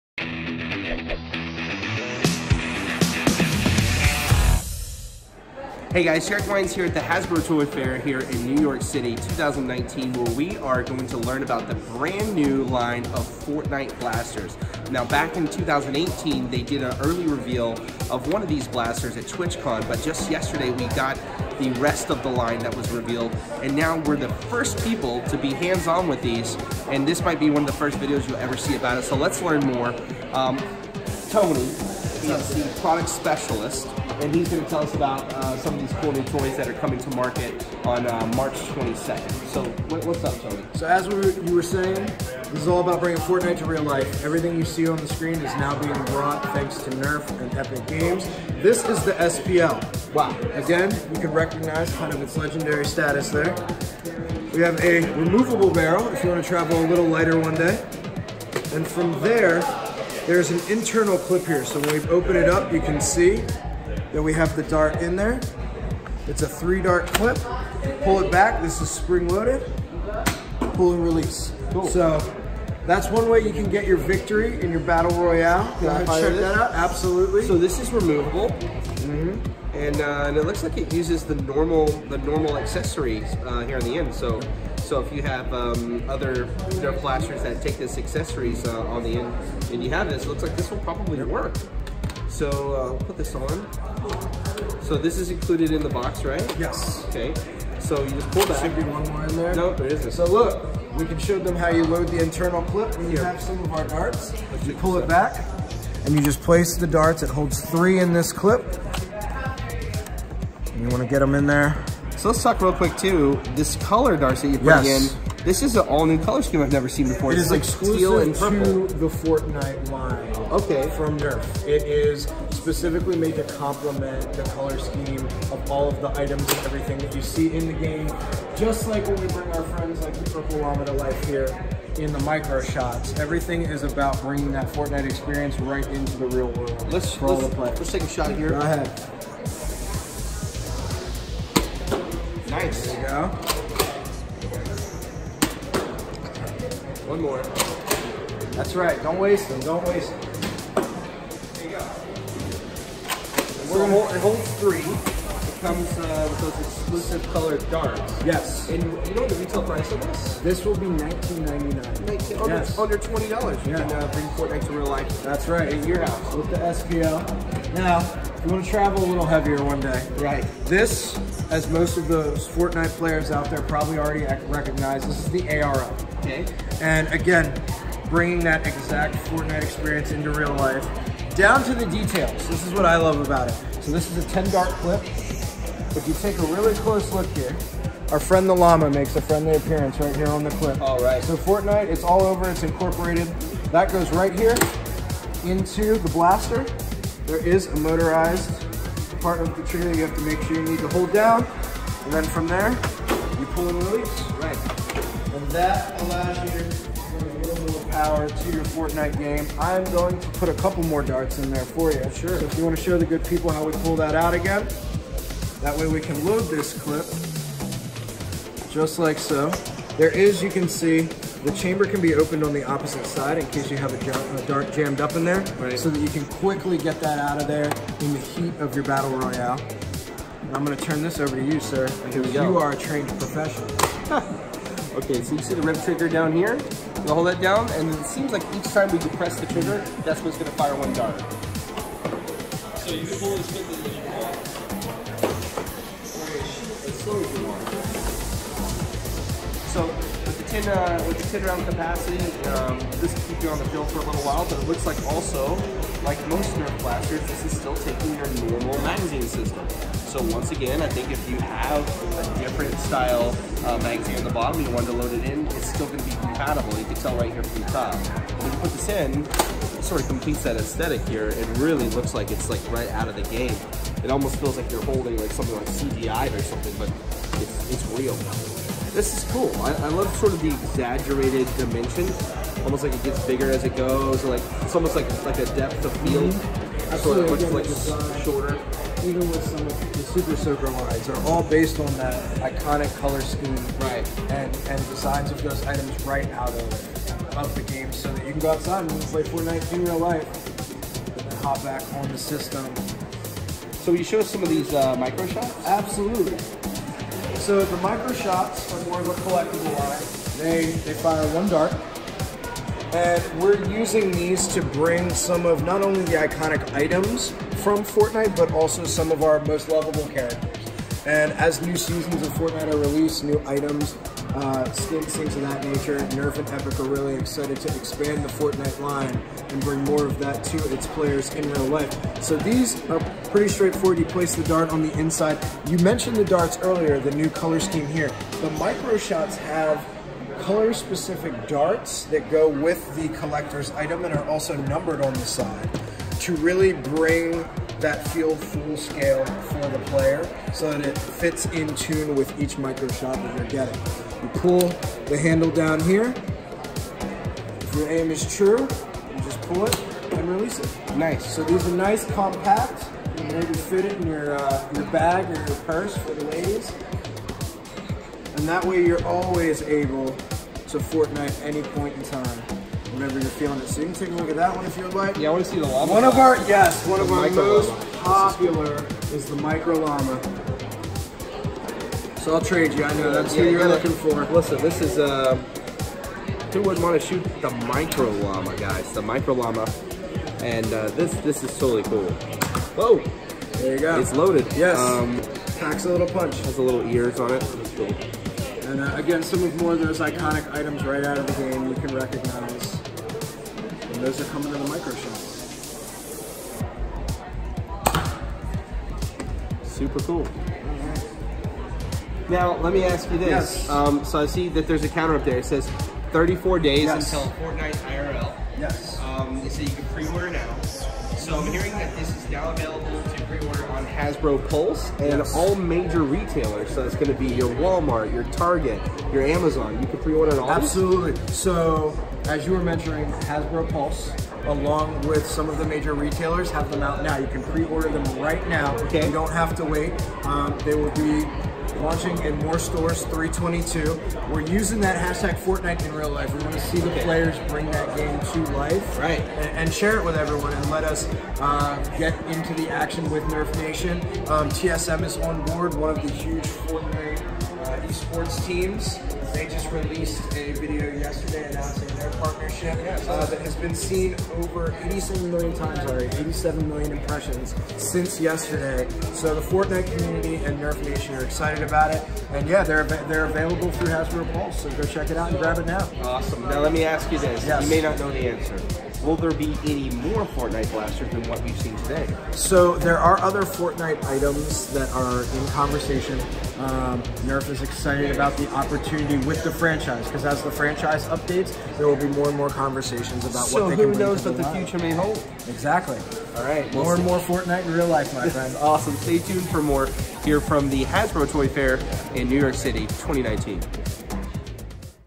[FIRST LOOK] Nerf Fortnite Blasters - Toy Fair 2019
This is your first look at the AR-L and SP-L blasters along with the Nerf Fortnite Microshots! Global availability is March 22nd 2019.